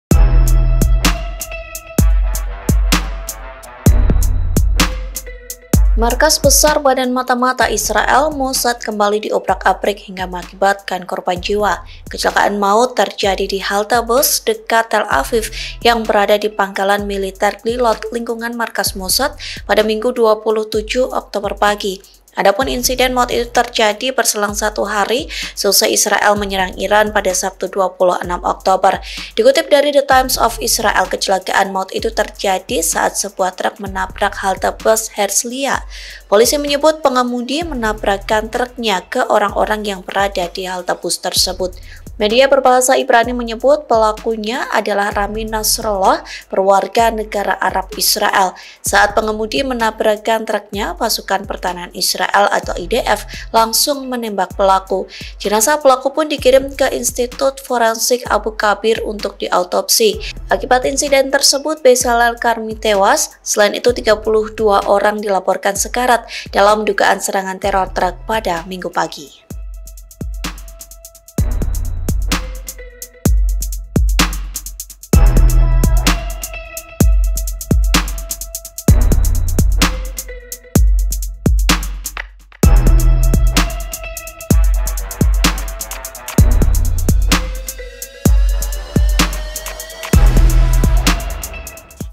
Markas Mossad Diobrak-abrik usai Serang Iran, Pasukan IDF Diseruduk Truk: 1 Tewas & 32 Orang Sekarat
BANGKAPOS.COM - Markas besar badan mata-mata Israel, Mossad kembali diobrak-abrik hingga mengakibatkan korban jiwa.

Kecelakaan maut itu terjadi di halte bus dekat Tel Aviv, yang berada di pangkalan militer Glilot, lingkungan Markas Mossad pada Minggu (27/10) pagi.

Adapun insiden maut itu terjadi berselang satu hari seusai Israel menyerang Iran pada Sabtu (26/10).